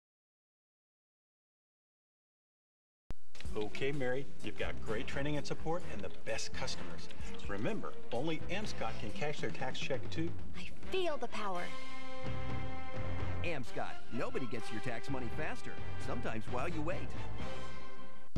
Greg's & Annie's Amscot
Filmed in Tampa in late 2007.  First time I've worked with Annie, and this Director and crew.  They are all great talents!  I'd love to work with them again.